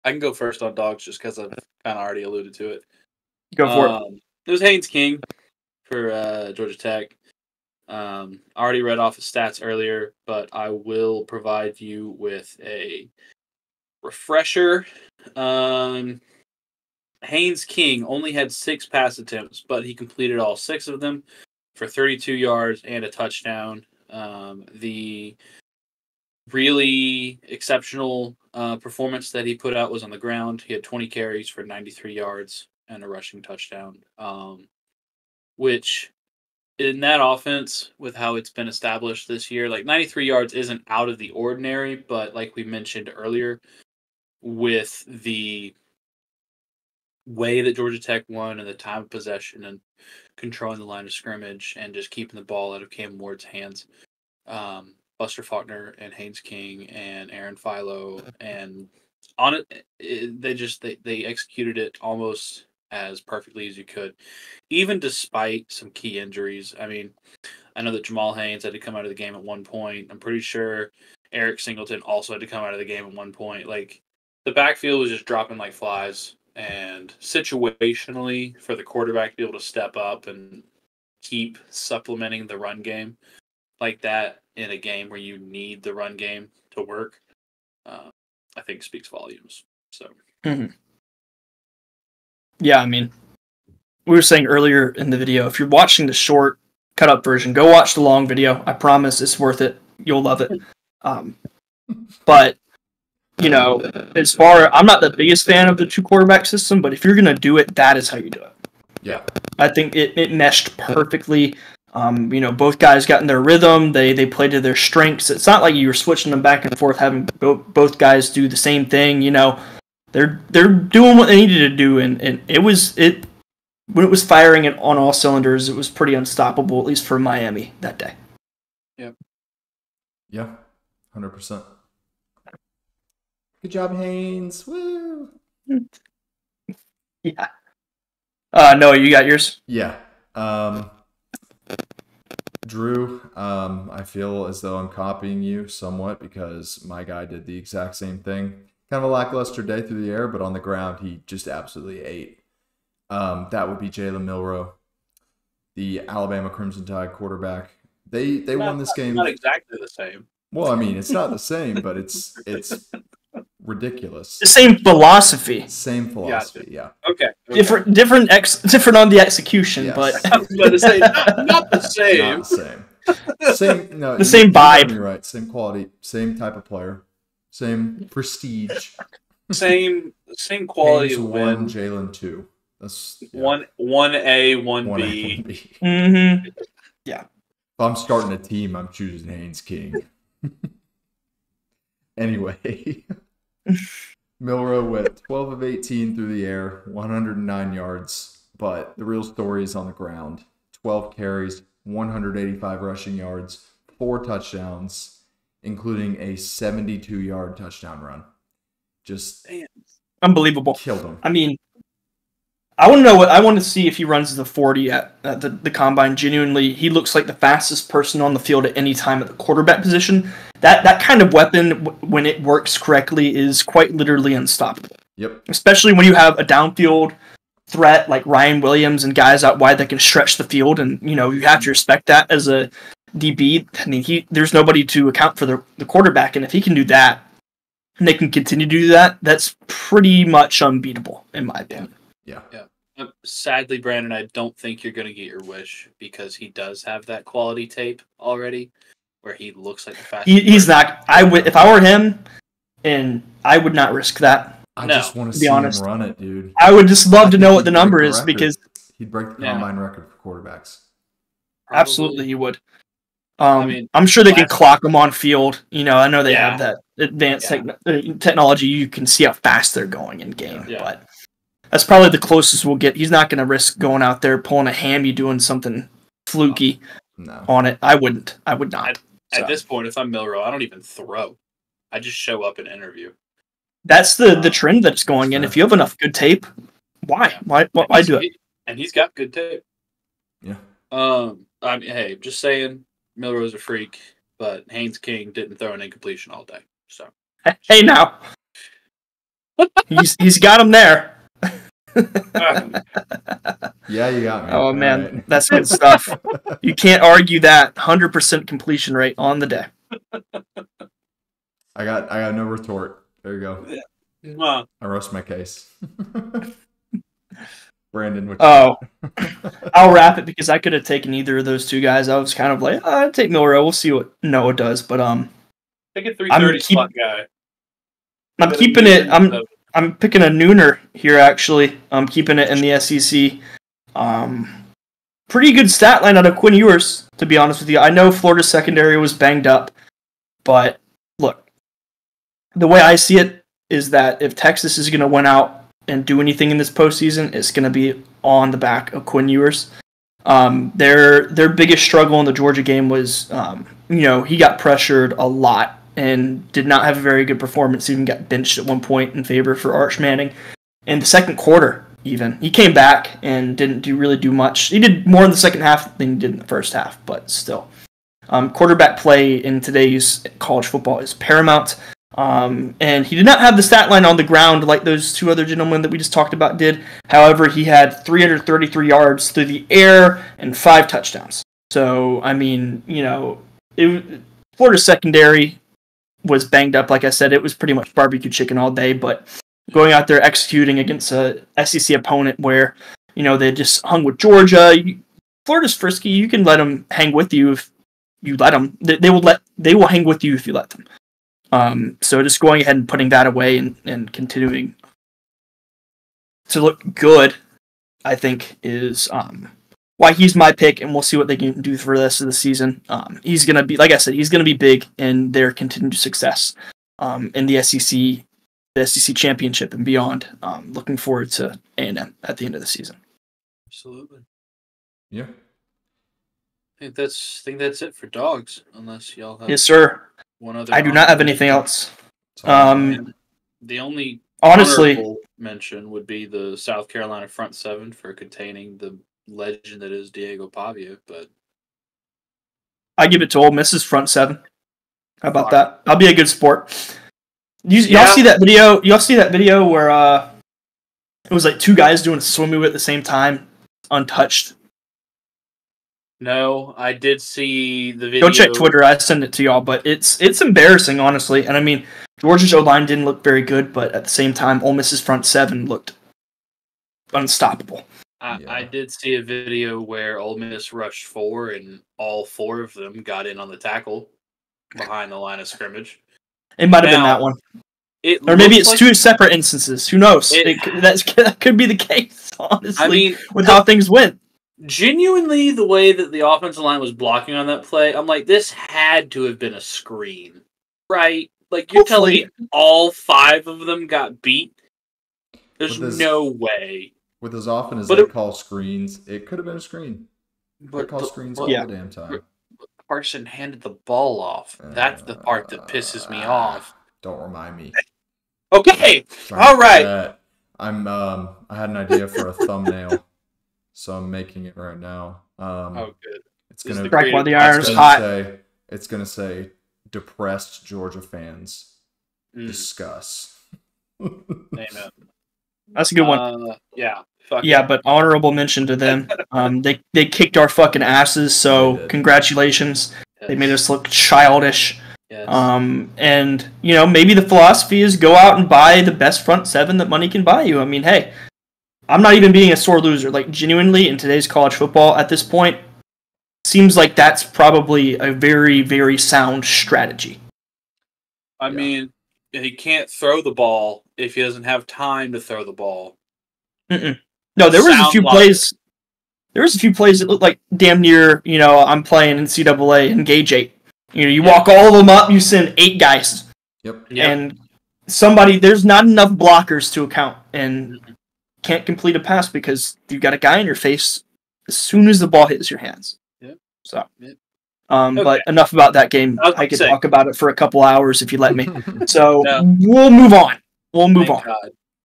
Is Jalen Milroe BACK in the HEISMAN Conversation? (Week 11 College Football Dawgs of the Week)
Welcome to 3 & Out!

Week 11 dawgs of the week are here! Let us know in the comments who YOUR choice for dawg of the week in week 11 of college football is and be sure to subscribe and stick around for college football content all season long!

Timestamps-
Haynes King: 0:00-2:20
Jalen Milroe: 2:21-5:17
Quinn Ewers: 5:18-7:12
Honorable Mentions: 7:13-9:44

Thanks for hanging out and talking ball!